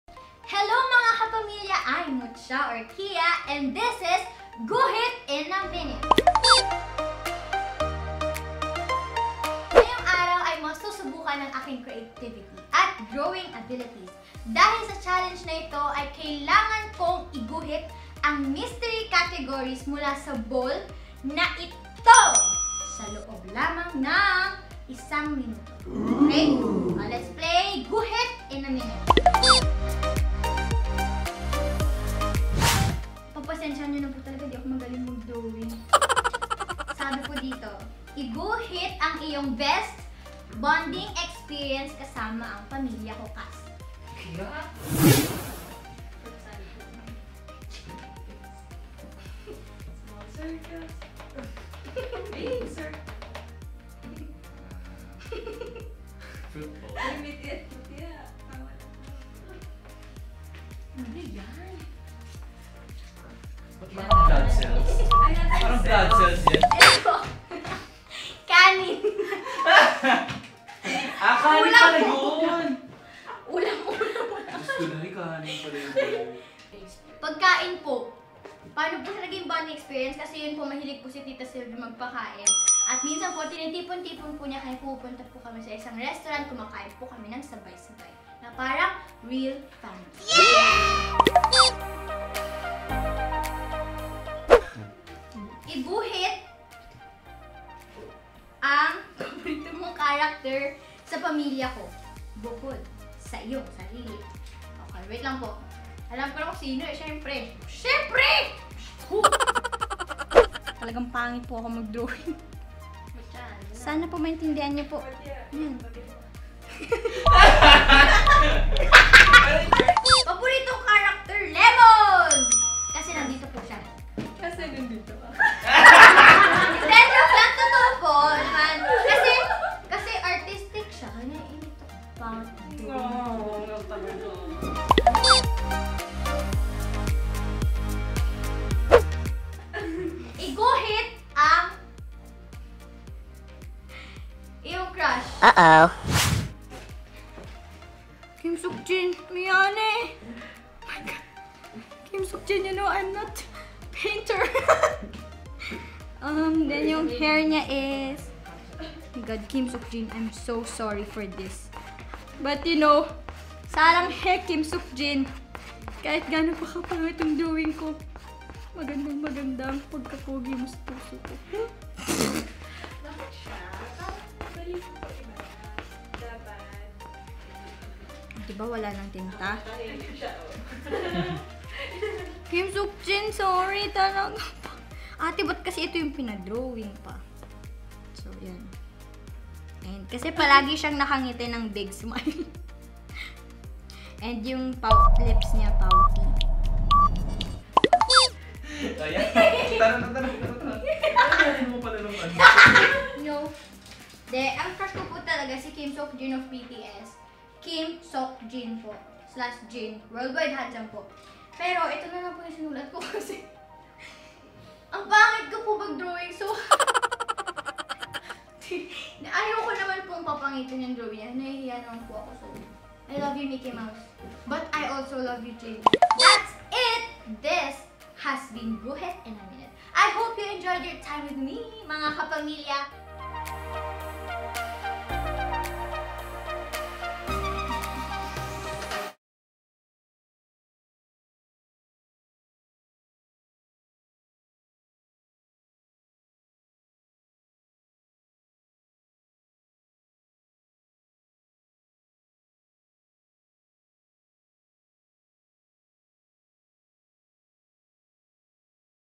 Guhit In A Minute with Mutya Orquia
Pamilya Ko star Mutya Orquia plays Guhit In A Minute.